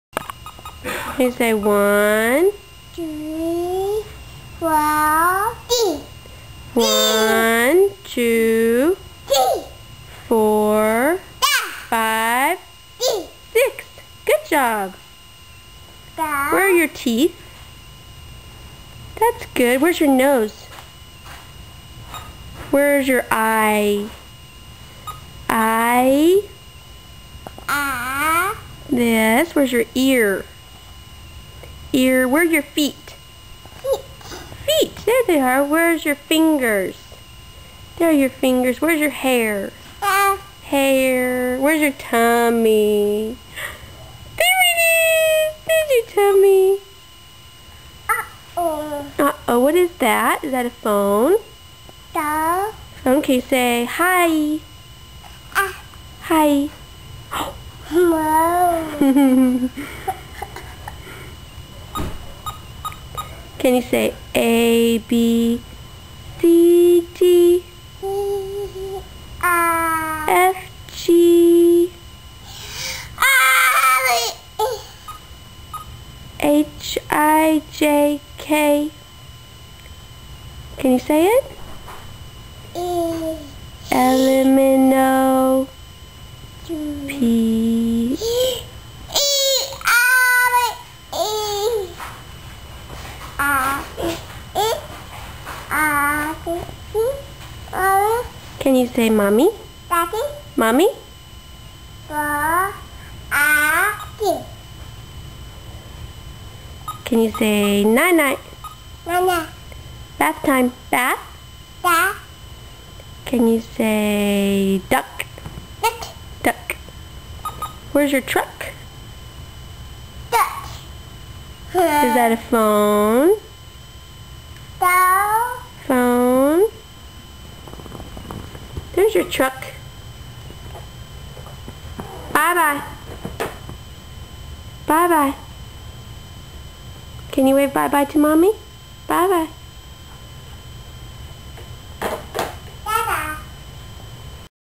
Talking to Mommy
Connor telling us what he knows.